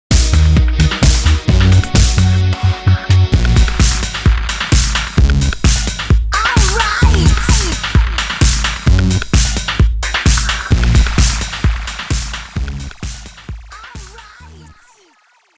Puma - Spec Animation
I created a spec graphics animation for Puma shoes.